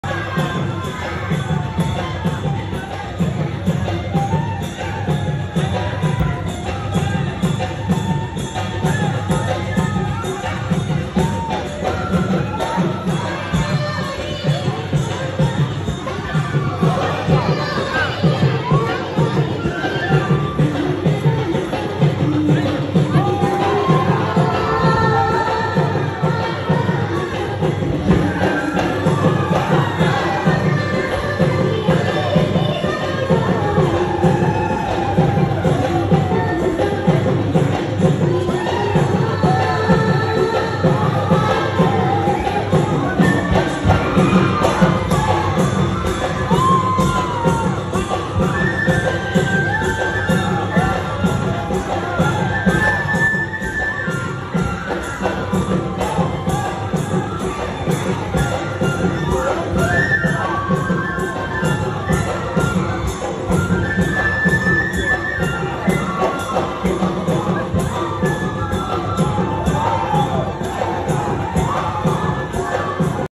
Dibrugarh University 🔥2023 Assam
Youth festival in digboi for the first time
Digboi mahila mahavidyalaya 2023